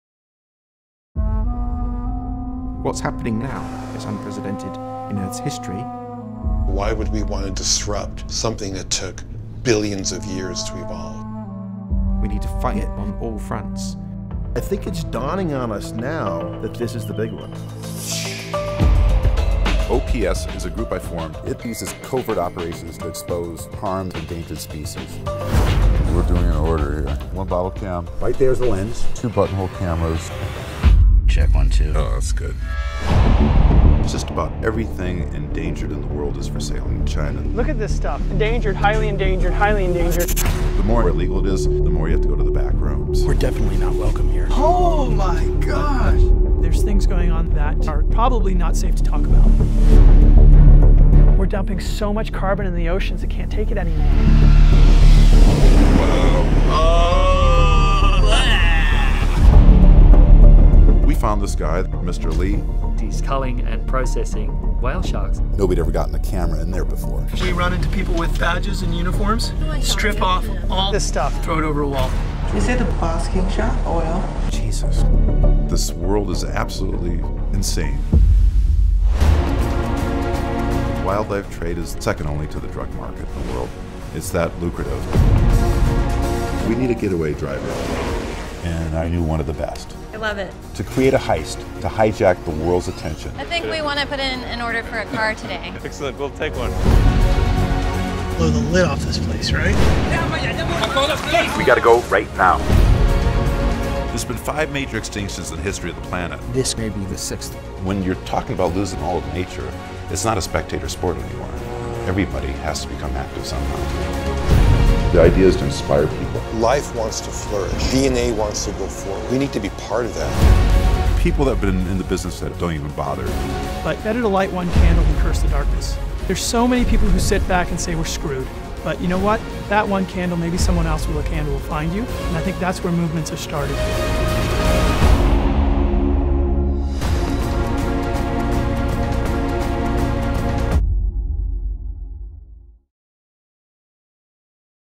Racing Extinction - Maui Film Festival
Hawaii Premiere of "Racing Extinction" at the 2015 Maui Film Festival on June 4 (Thursday) 10:00pm Celestial Cinema, Wailea.